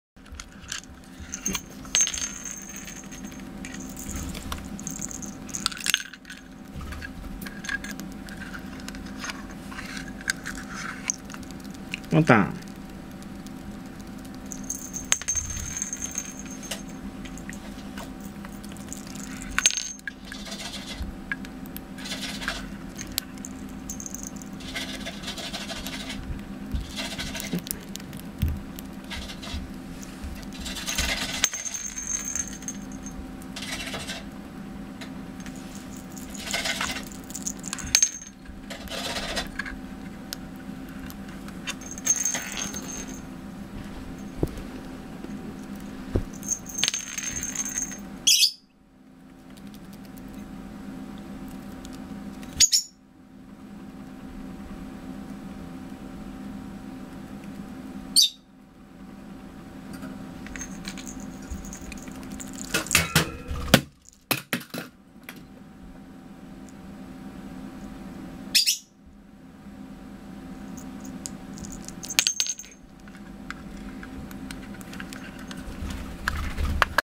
LoveBird りんりんぽんぽん＠ぽんたろう（コザクラインコ）
■おぴ～＠コザクラインコ（・Θ・）ぎゅ

預かりインコのポンへのリンリントレーニング。
２～３０分でこれぐらいを習得（＾Θ＾）